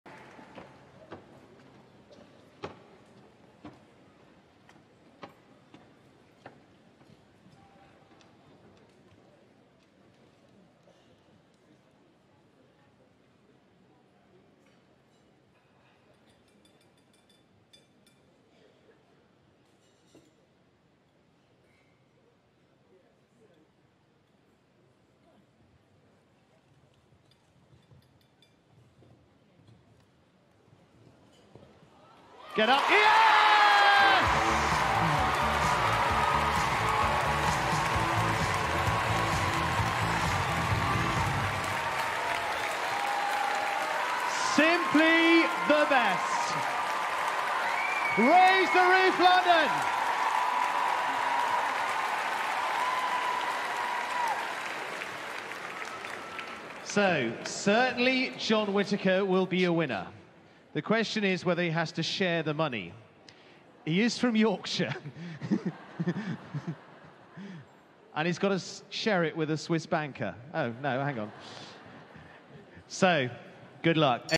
John Whitaker & Sharid Win The Six Bar | London International Horse Show
The man, the myth, the legend - At 68 years young John Whitaker jumps 1.90m on Sharid in perfect style to win the Six Bar at The London International Horse Show👏🏼🥇

Watch London International Horse Show back on demand on H&C+ 🎥